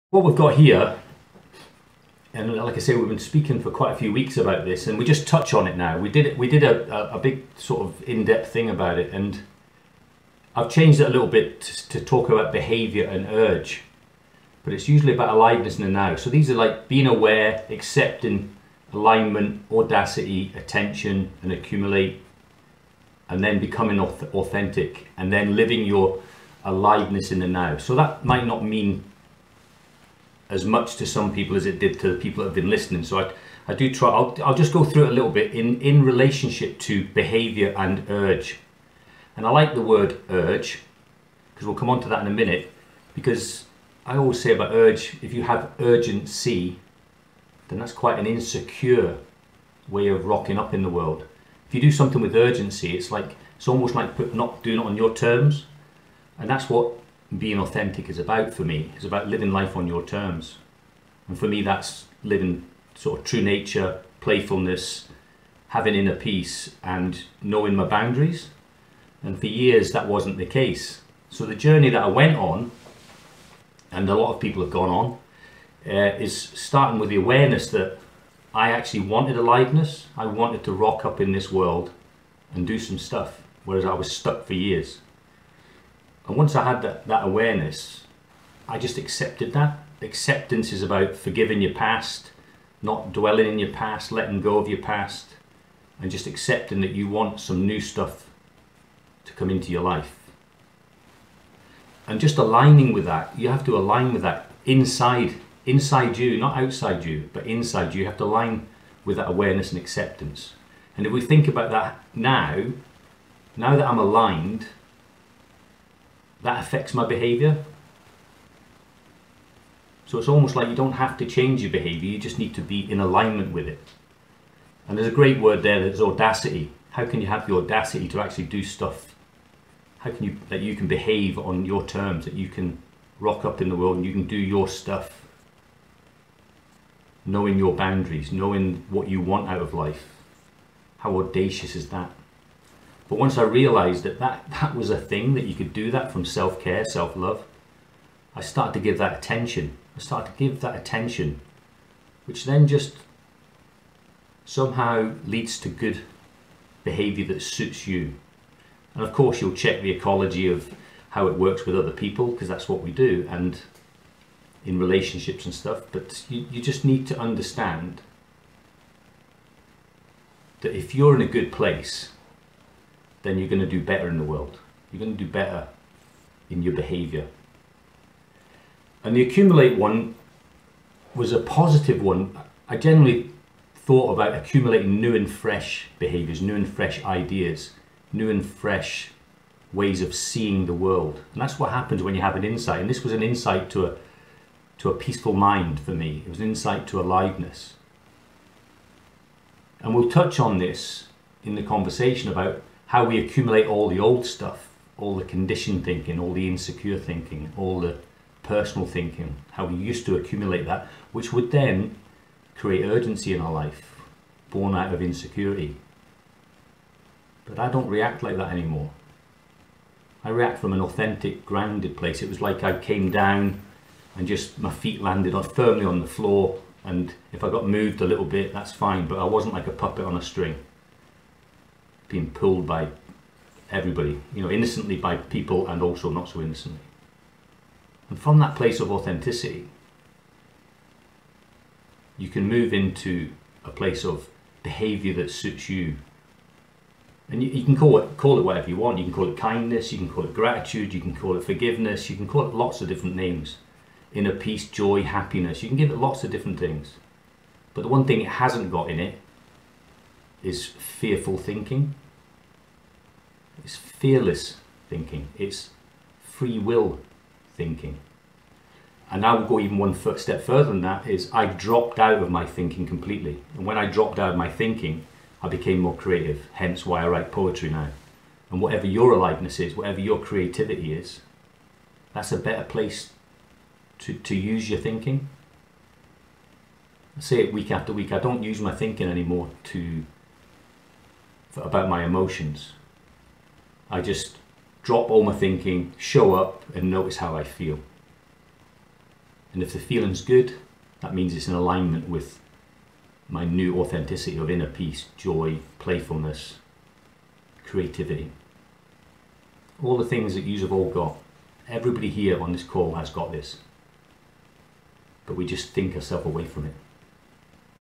How to change our behaviour to help us with our True Nature
The 7''As to an Authentic aliveness in the now creates a better way of showing up in the world.